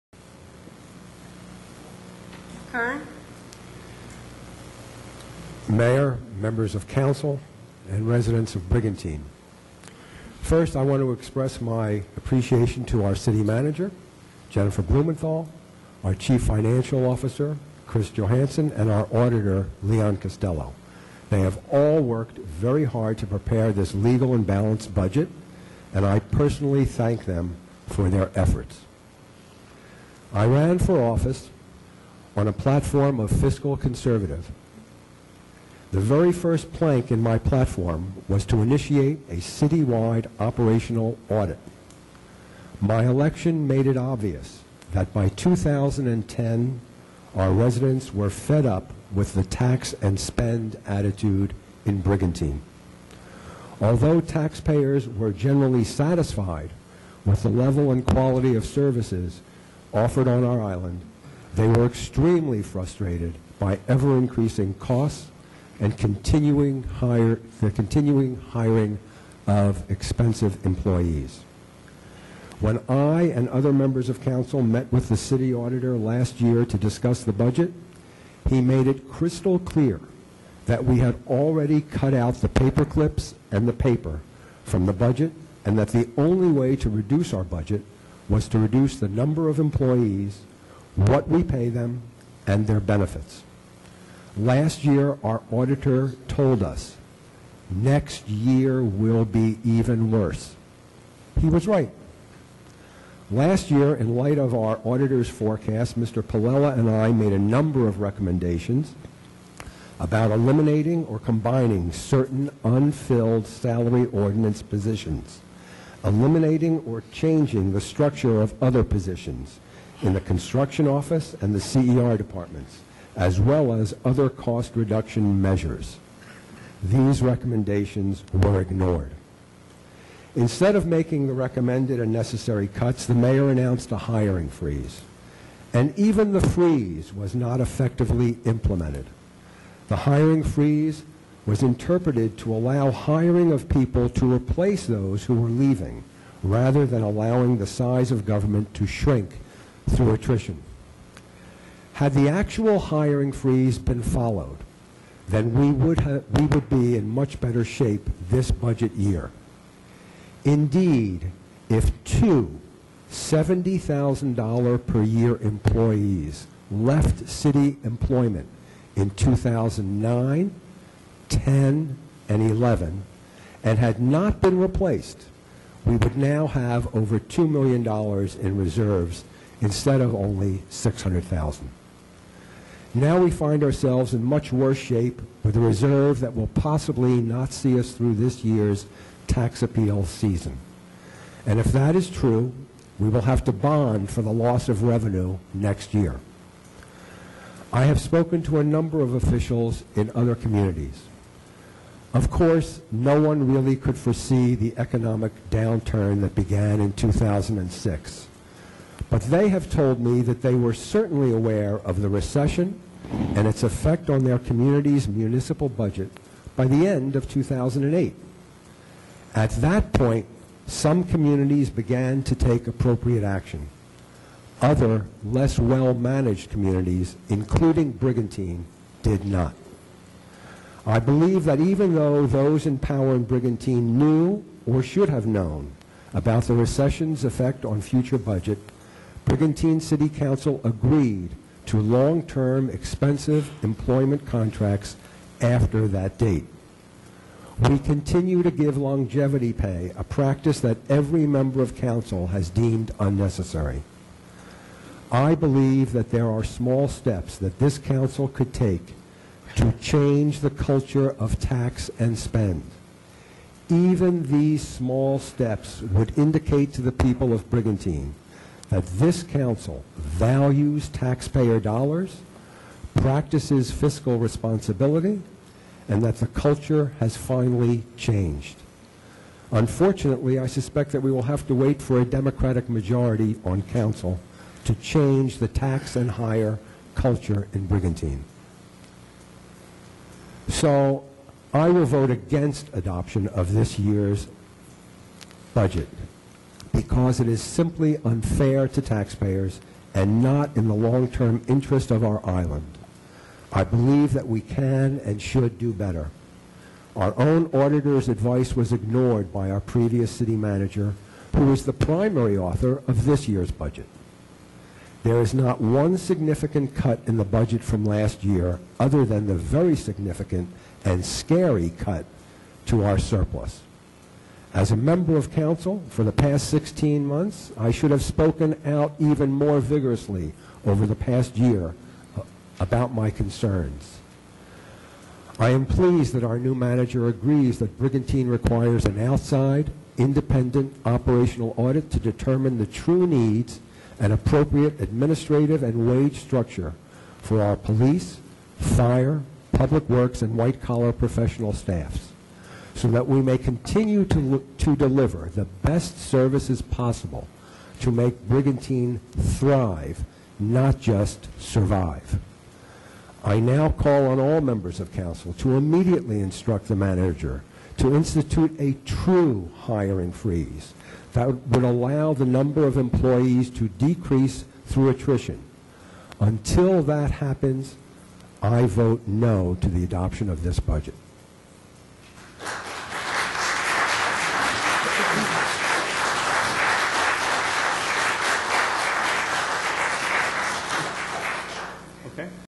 DrKernBudgetComments041812
Dr. Frank Kern, Councilman at Large gives his assessment of the city's budget problems and solutions.